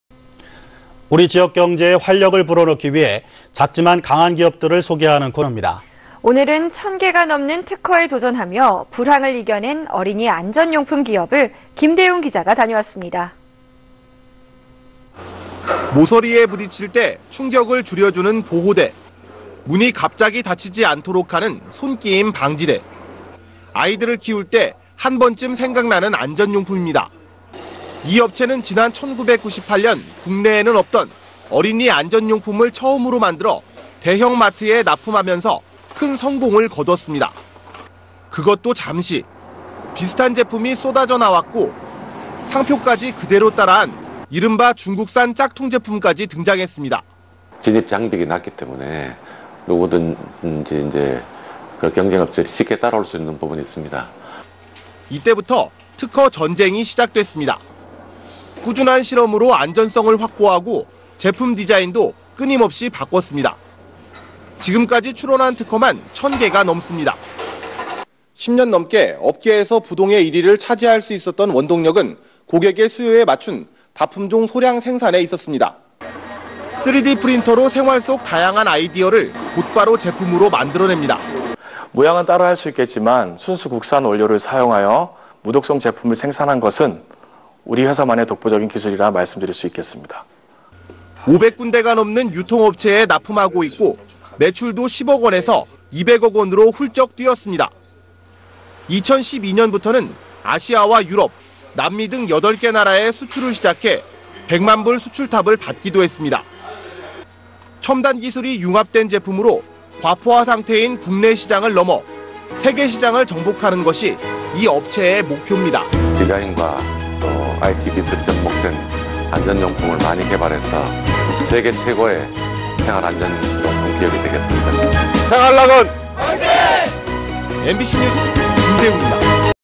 [청주MBC뉴스] 주식회사 '생활낙원'
◀앵커▶
 지역경제에 활력을 불어넣기 위해
작지만 강한 기업들을 소개하는 코너입니다.
 오늘은 천 개가 넘는 특허에 도전하며
불황을 이겨 낸 어린이 안전용품 기업을
김대웅 기자가 다녀왔습니다.
           ◀ＥＮＤ▶

           ◀ＶＣＲ▶
 모서리에 부딪힐 때 충격을 줄여주는 보호대,
문이 갑자기 닫히지 않도록 하는
손끼임 방지대.

 아이들을 키울 때 한번 쯤 생각나는
안전용품입니다.

 이 업체는 지난 1998년 국내에는 없던
어린이 안전용품을 처음으로 만들어
대형마트에 납품하면서 큰 성공을 거뒀습니다.

 그것도 잠시, 비슷한 제품이 쏟아져 나왔고
상표까지 그대로 따라한 이른바
중국산 짝퉁 제품까지 등장했습니다.

           ◀ＩＮＴ▶
김기석/(주) 생활낙원 대표이사
"진입 장벽이 낮다보니 경쟁 업체 난립"

 이 때부터 특허 전쟁이 시작됐습니다.

 꾸준한 실험으로 안전성을 확보하고,
제품 디자인도 끊임없이 바꿨습니다.

 지금까지 출원한 특허만 천 개가 넘습니다.

(s/u) 10년 넘게 업계에서 부동의 1위를
차지할 수 있었던 원동력은 고객의 수요에 맞춘
다품종 소량 생산에 있었습니다.

 3D 프린터로 생활 속 다양한 아이디어를
곧바로 제품으로 만들어 냅니다.

           ◀ＩＮＴ▶
장동일/(주)생활낙원
"국내산 원료 무독성 제품 국내에서 독보적"

 5백 군데가 넘는 유통업체에
납품하고 있고, 매출도 10억 원에서
2백억 원으로 훌쩍 뛰었습니다.

 2012년부터는 아시아와 유럽,
남미 등 8개 나라에 수출을 시작해
백만 불 수출탑을 받기도 했습니다.

 첨단 기술이 융합된 제품으로
과포화 상태인 국내 시장을 넘어
세계 시장을 정복하는 것이
이 업체의 목표입니다.

           ◀ＳＹＮ▶
"생활 낙원, 화이팅"